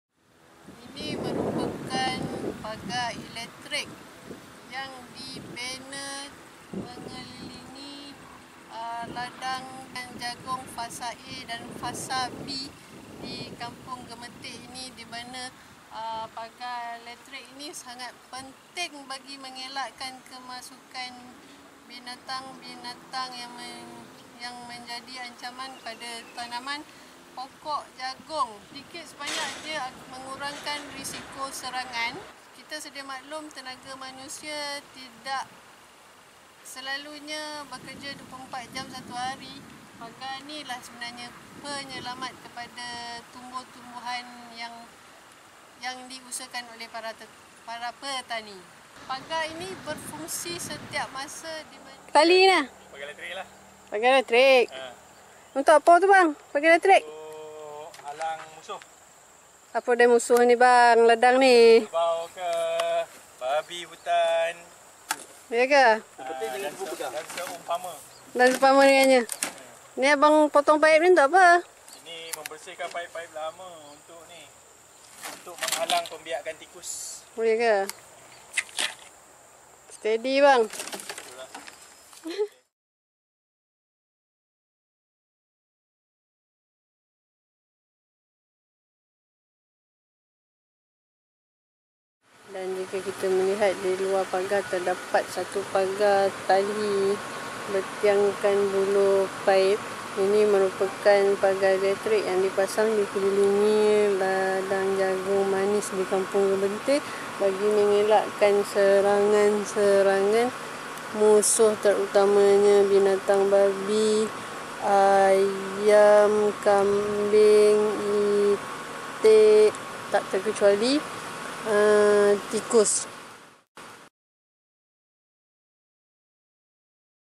VIDEOMENTARI PERTANIAN I APA FUNGSI PAGAR ELEKTRIK DI LADANG JAGUNG?
Bukan mudah untuk menjaga tanaman jagung jika tidak dipagar. Pagar pula bukan setakat pagar dawai untuk meletakkan sempadan ladang agar tidak masuk ke tanah orang.
Ada satu lagi pagar amat penting diusahakan di ladang yang berskala besar.
Mahu tahu kenapa perlu pagar elektrik di ladang???
Jom vitea!
Terima kasih!